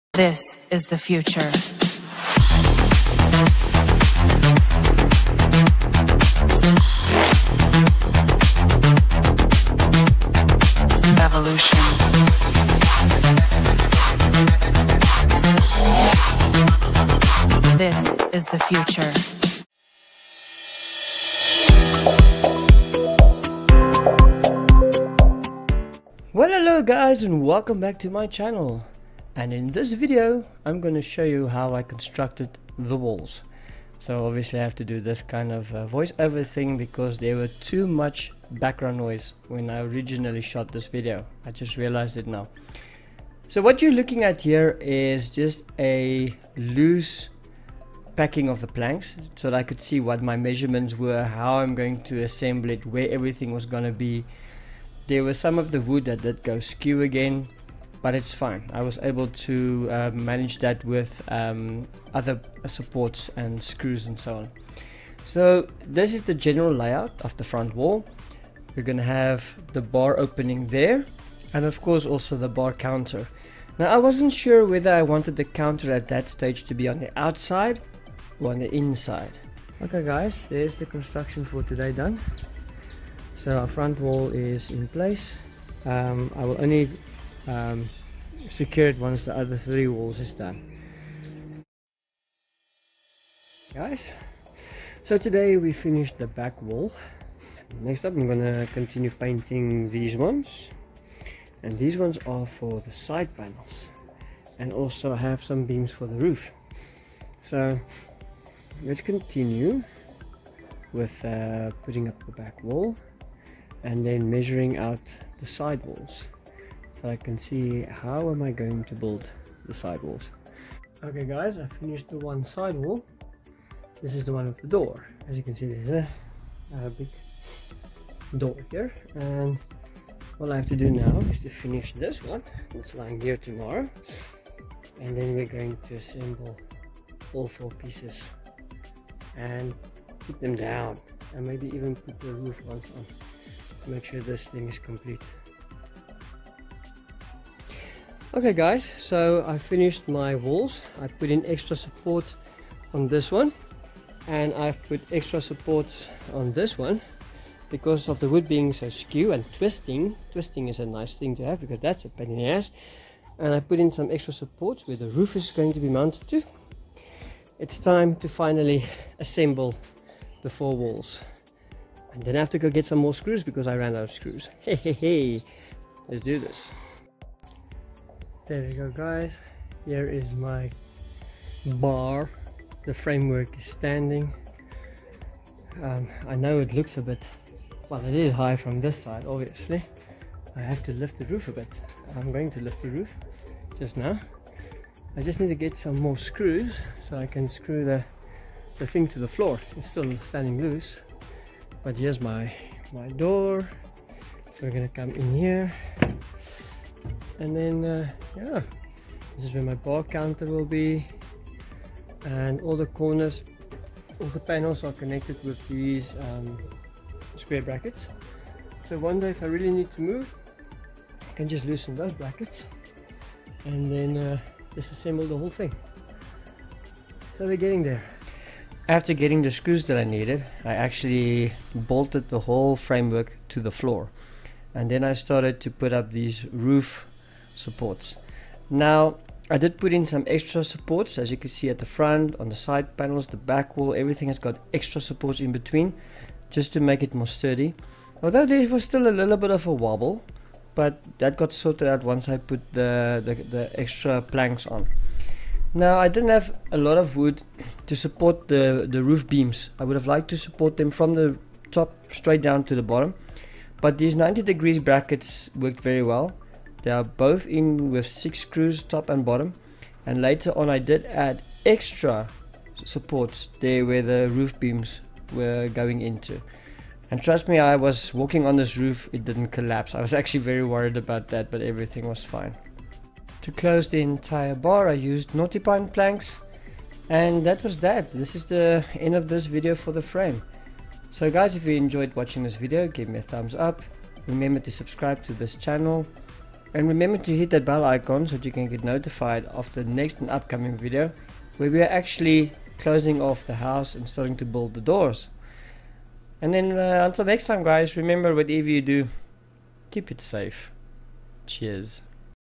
Tiki bar Project. Constructing the walls.
Hi guys. Here is part 2 of how I constructed my bar. In this video you will see how I made the wall frames.